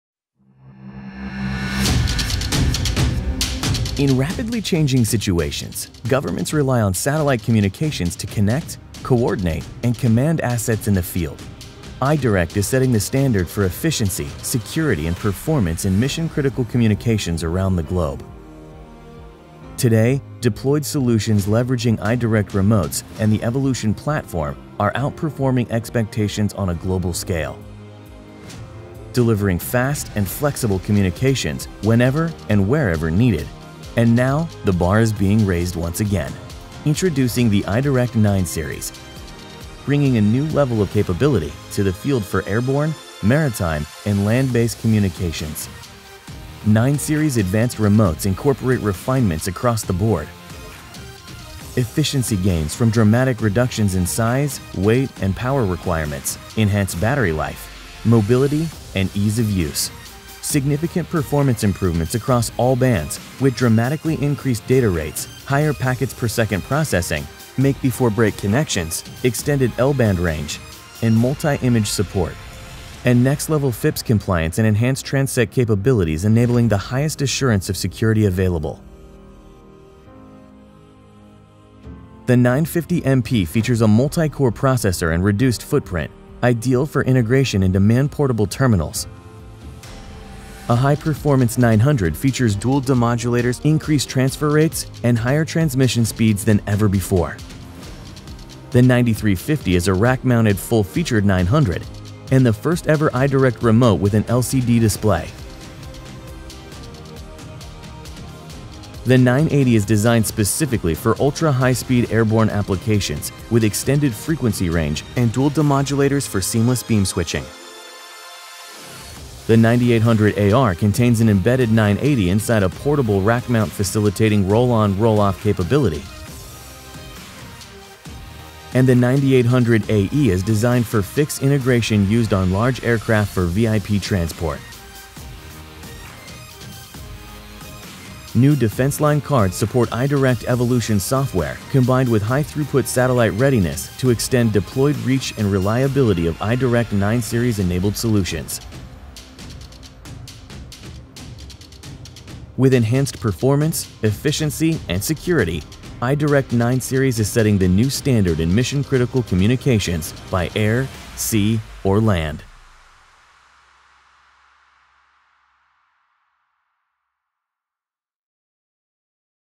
iDirect 9-Series Remotes
Introducing the iDirect 9-series remotes — bringing a new level of capability to the field for airborne, maritime and land-based communications.